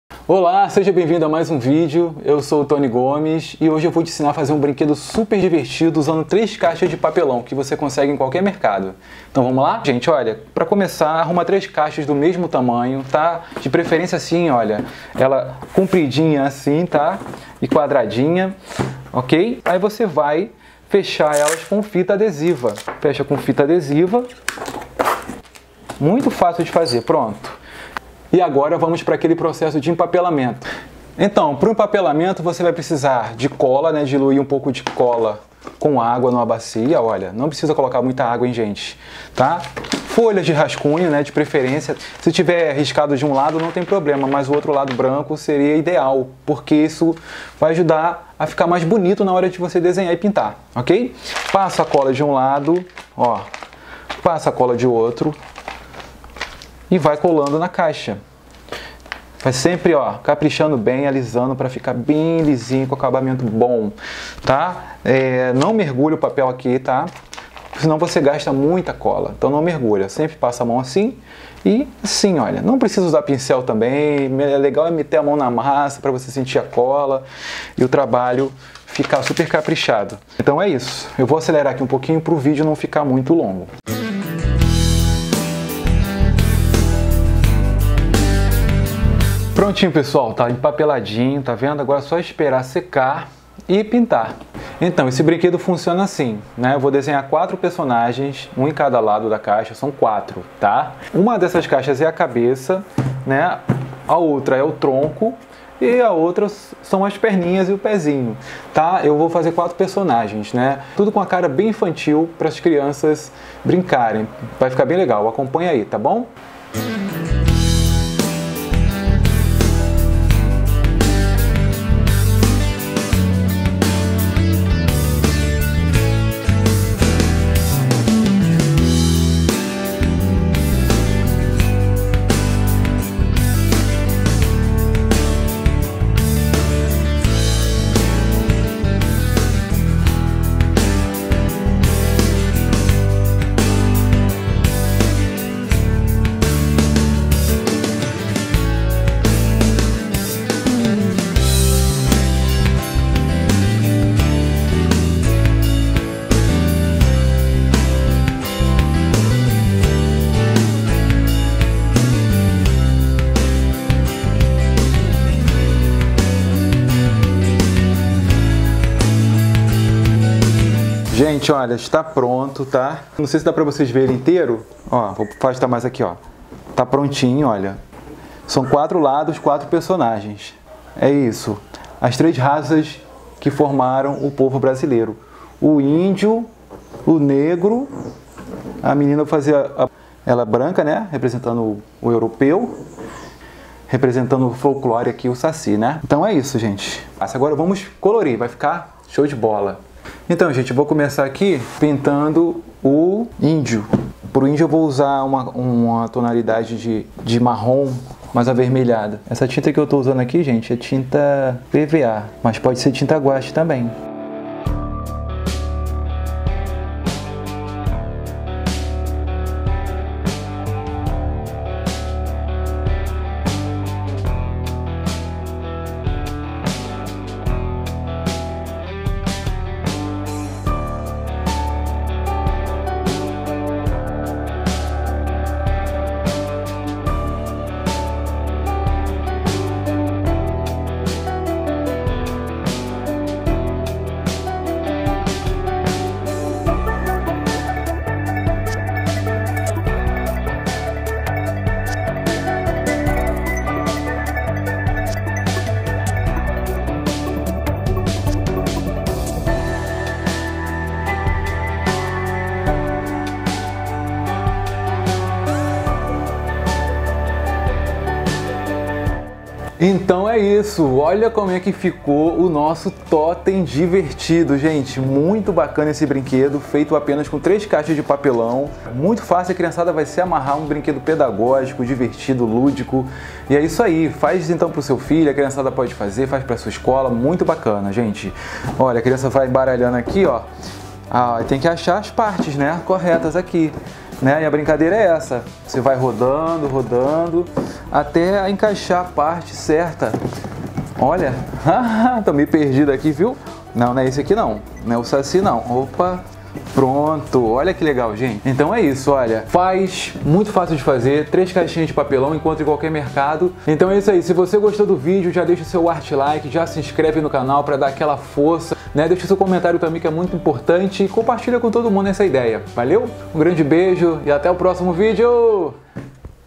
Ideia INCRÍVEL com CAIXAS de PAPELÃO | TOTEM DIVERTIDO
Nesse vídeo eu vou te ensinar a fazer um brinquedo incrível com caixas de papelão, eu chamo de   "TOTEM DIVERTIDO" a criançada vai amar!

Fale comigo no insta.: @tonygomesarte